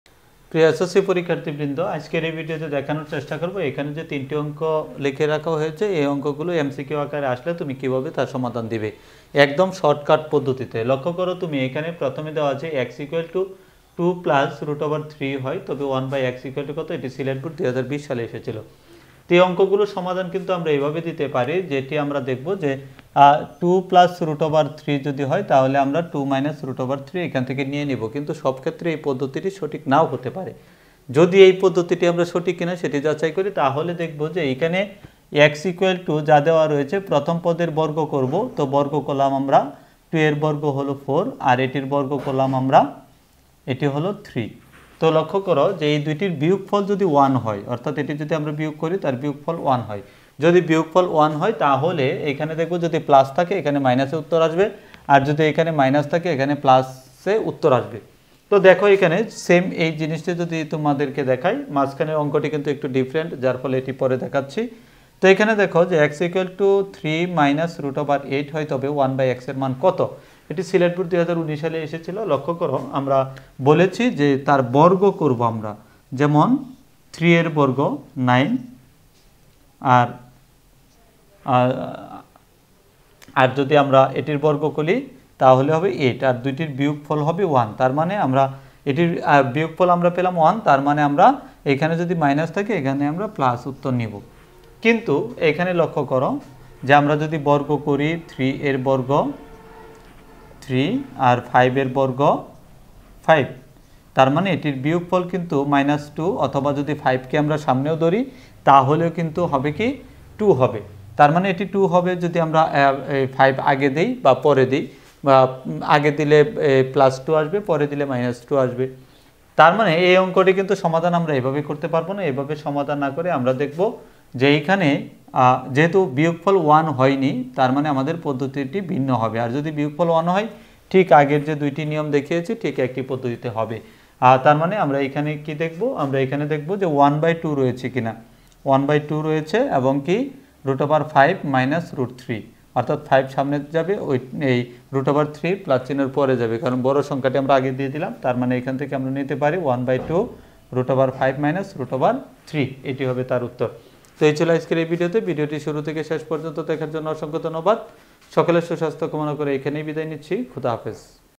যেকোনো বীজগণিতের মান নির্ণয় দেখা মাত্রই উত্তর দেওয়ার কৌশল | ssc | Algebra math | MMM education class
এসএসসি গণিত বহুনির্বাচনি বীজগণিতের মান নির্ণয় কিভাবে দেখা মাত্রই উত্তর দিতে পারবে তা জেনে নাও।